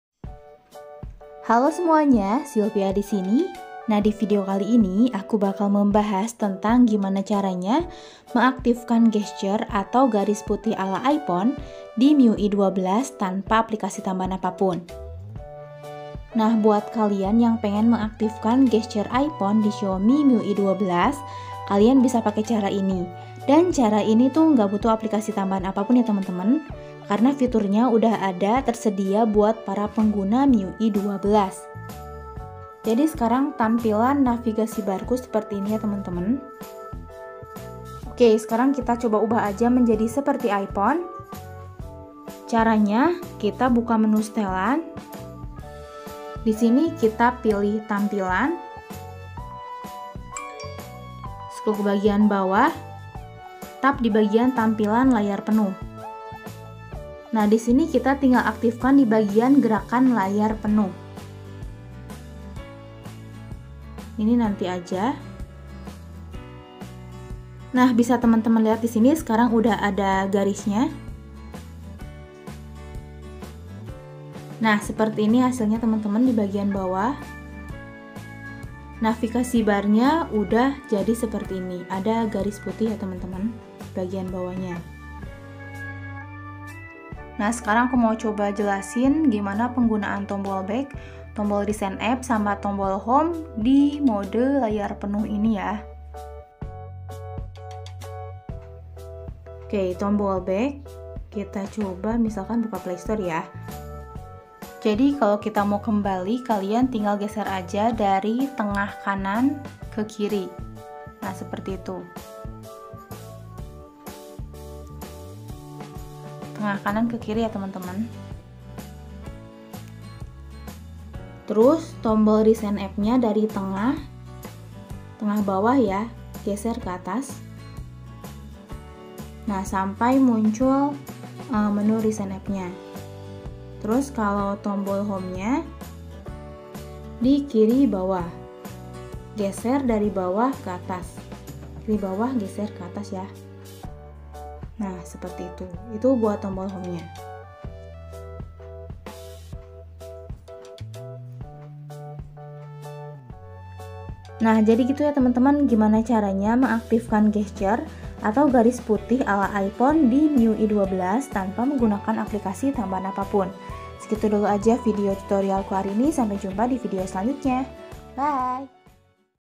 Cara Mengaktifkan Gesture Iphone Di MIUI 12 Tanpa Aplikasi
Cara Mengaktifkan Gesture Iphone Di MIUI 12 Tanpa Aplikasi - Di Xiaomi MIUI 12 sekarang ini kita sudah bisa mengubah tampilan layarnya menjadi tampilan layar penuh dengan gesture yang mirip dengan iphone. Kita bisa mengaktifkan gesture iphone di MIUI 12 tanpa harus menggunakan aplikasi tambahan apapun karena fiturnya sudah tersedia di pengaturan bawaan dari hp xiaomi MIUI 12. Selengkapnya lihat di artikel berikut ini